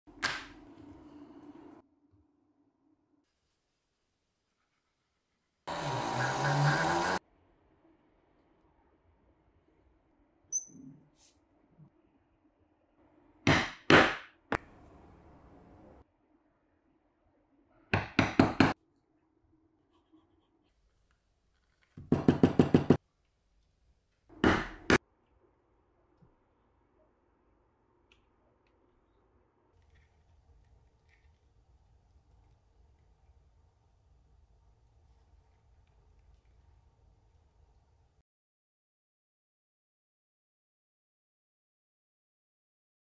How expensive Italian leather wallet is made?
Wallet is made of "Ponza" and "Mexico" Italian vegetable tanned leathers.
Totally 100% handmade and handstitched.